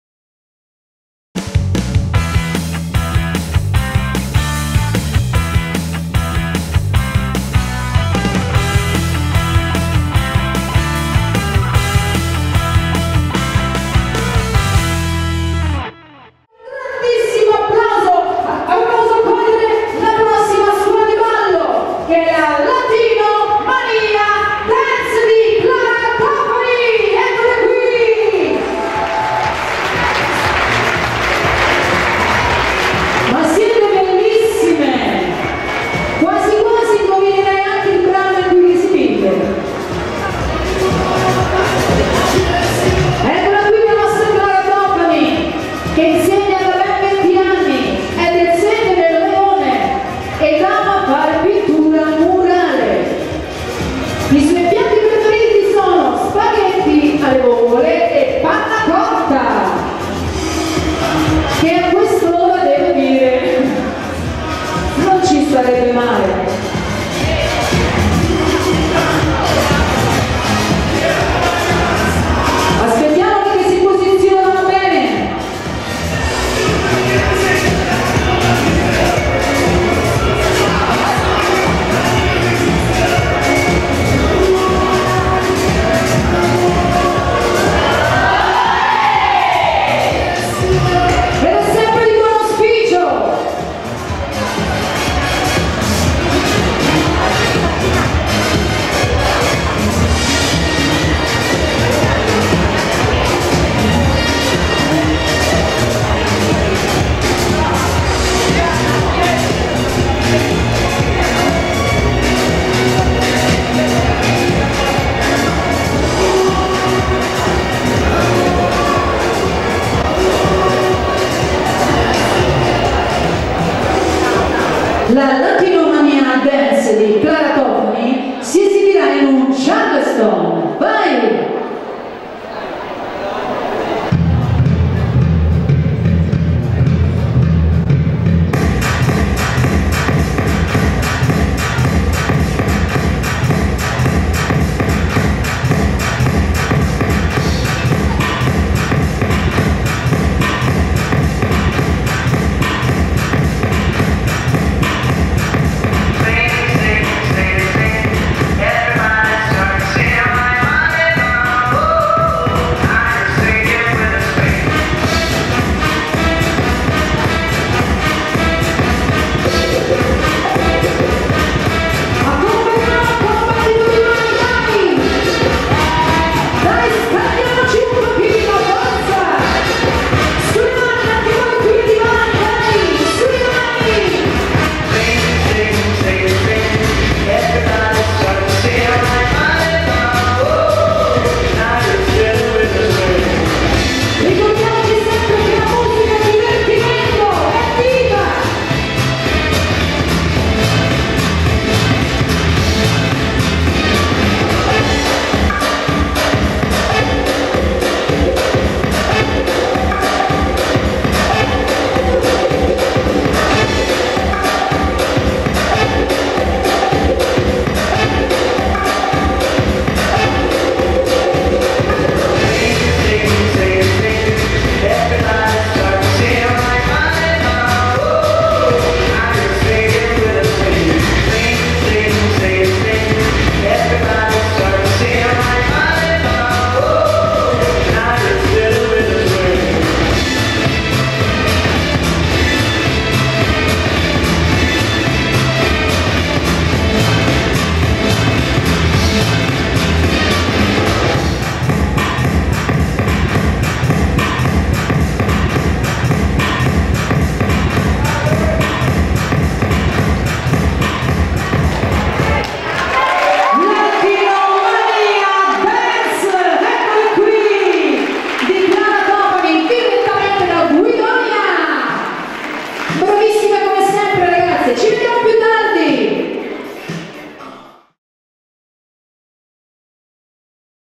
Ballo di Gruppo Charleston da Latinomania Dance coreografia di Clara Tofani 1° Raduno Palacavicchi
Da Ballo Italia il  1° Raduno Nazionale a Roma Palacavicchi un stupenda coreografia  con abiti  a tema  .